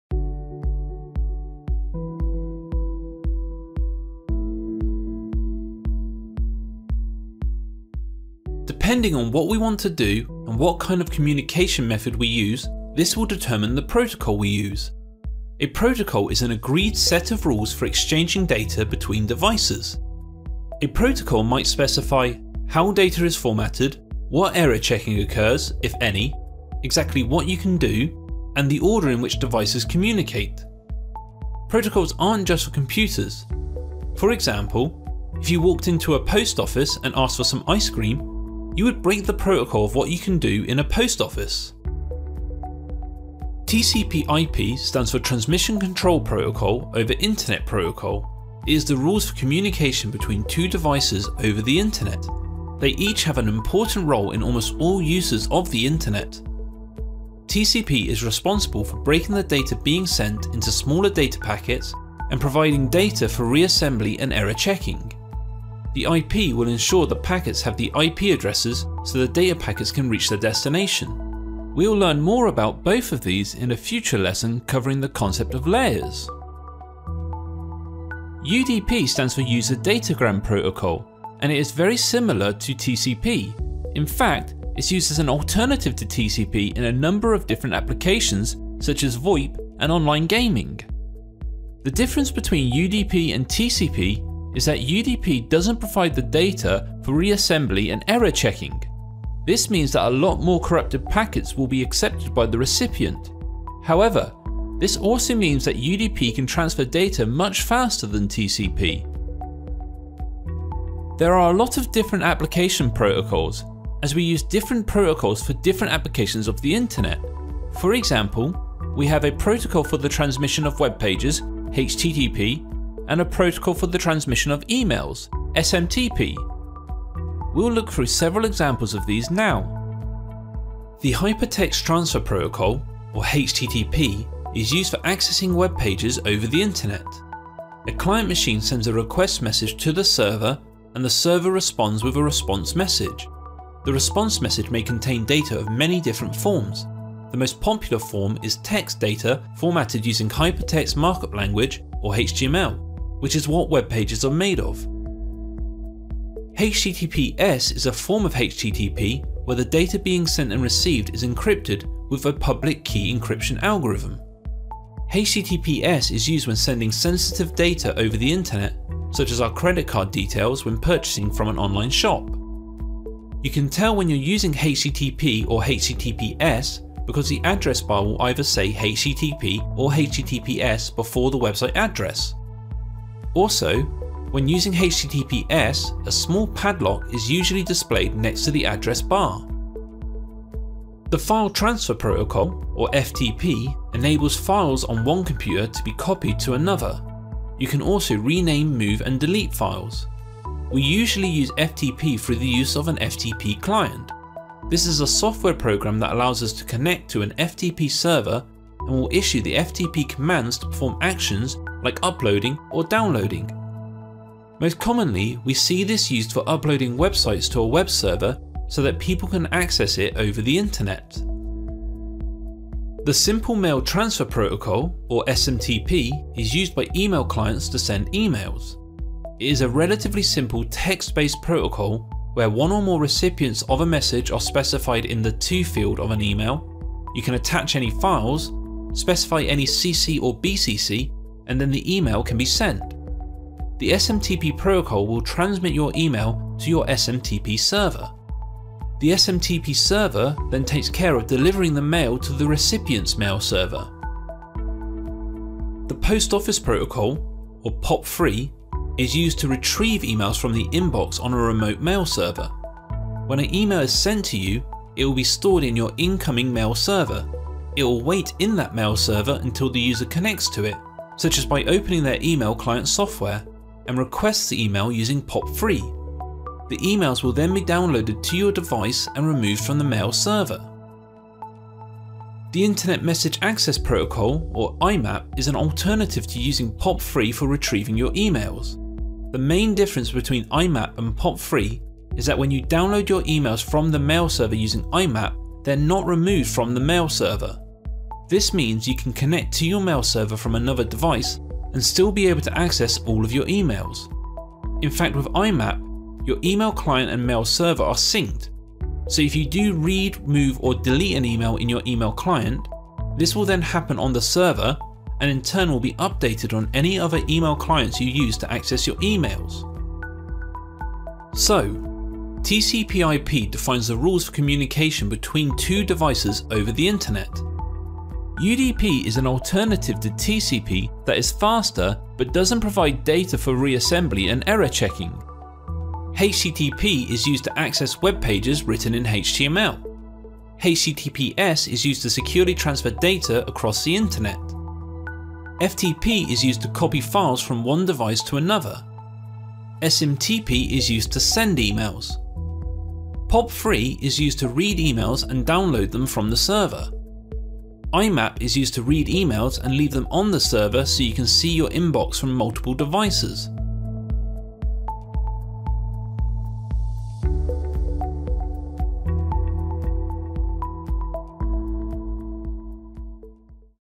Common Protocols - AQA GCSE Computer Science
Learn about common network protocols for your AQA GCSE Computer Science revision. You can access even more GCSE Computer Science study materials on the e-learning course

Specification:
AQA GCSE Computer Science (8525)
3.5 Fundamentals of Computer Networks